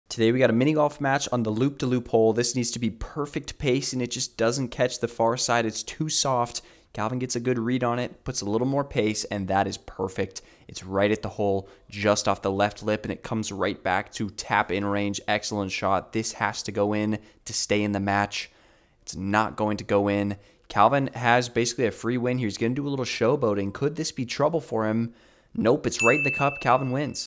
Is This The Hardest Obstacle in Mini Golf??
The Loop De Loop hole might be the hardest mini golf hole to make an ace! Can we do it?